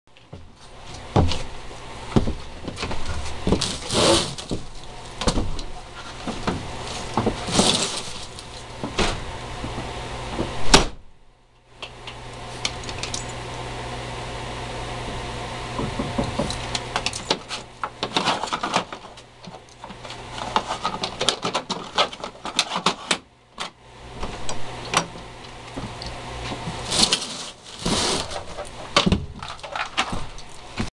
A dog opens doggie door
Skippy has learned how to open a dog door when it is closed, but not latched.